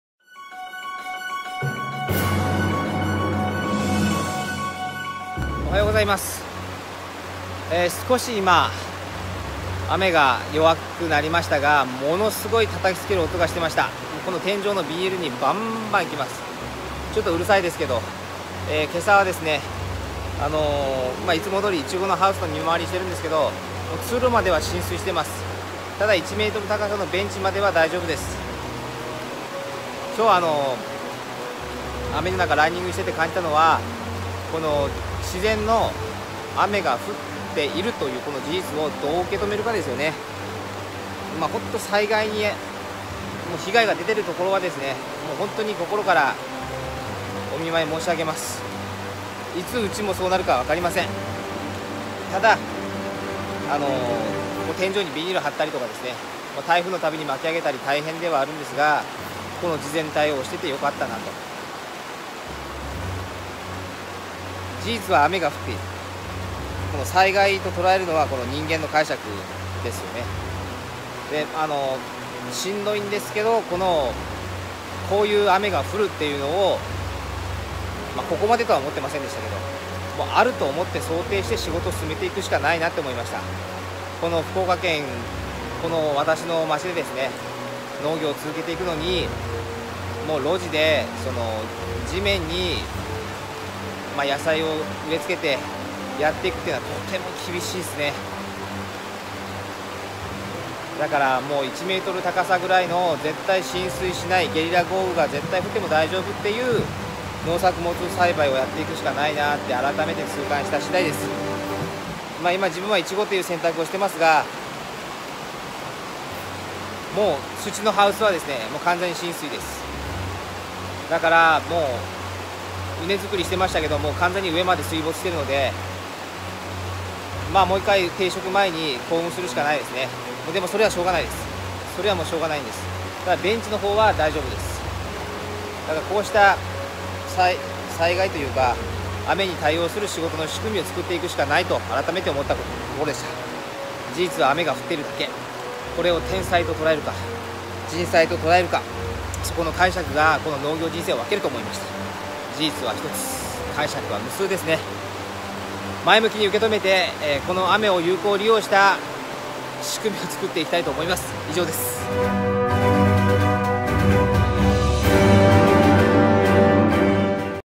事実は雨が降っているだけ
【雨が降っていることをどう解釈するか、】

自分が災害に合っていたらと思うともうとても苦しいし胸が痛みます。

被害が出ている方には心からお見舞い申し上げます。

でもこれから先この地域で農業していくには、
こういう天気が当たり前なんだと

そう思って事前対応していくしかないなと改めて感じました。

当たり前の基準をどこに置くか。

12月にお歳暮の予約がたくさんいただきます。
それに対する責任がなんとしても守るんだと言う
事前対応の作業を後押ししてくれるんです。

それでも自然の脅威になすすべがない時があります。

被害に遭われた方には心からお見舞い申し上げます。

うちもこれから先いつ同じ目に合うかわかりませんが、すべてを人災だと思って事前対応で受け入れていくしかないと思っています。

この事実をプラスに持っていける
仕事の仕組みを作っていきたいと思います。